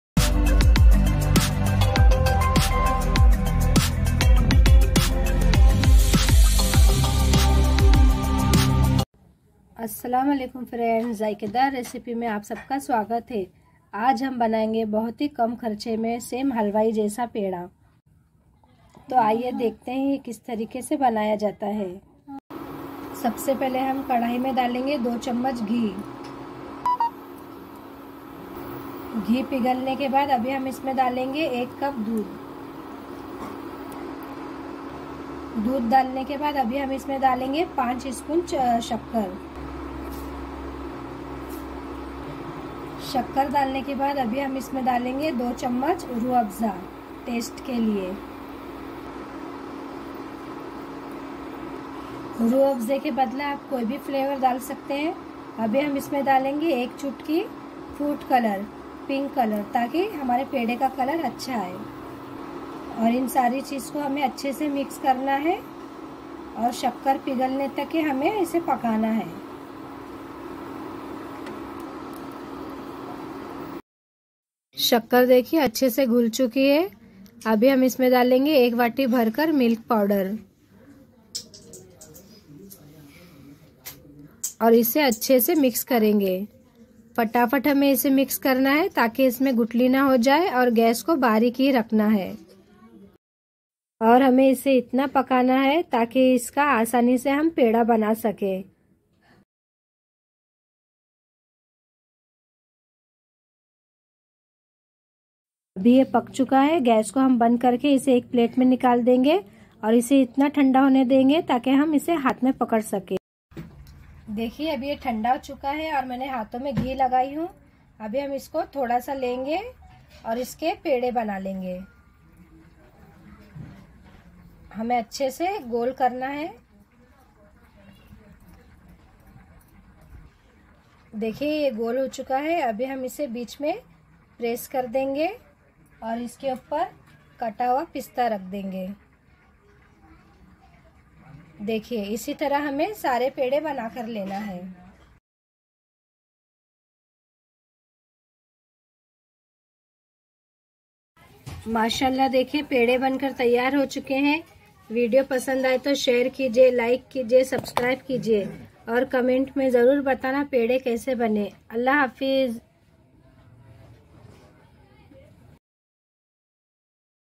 मिल्क पावडर और रूह अफजाह से आसान तरीकेसे बनाए हलवाई जैसा पेंढा..milk powder pedha..sweet recipe..
ingredients.

1.milk powder 1 vati
2.ghee 2spoon
3.sugar 5 spoon
4.rooh afza 2spoon
5.pink food colour
6.milk 1 cup
7.pista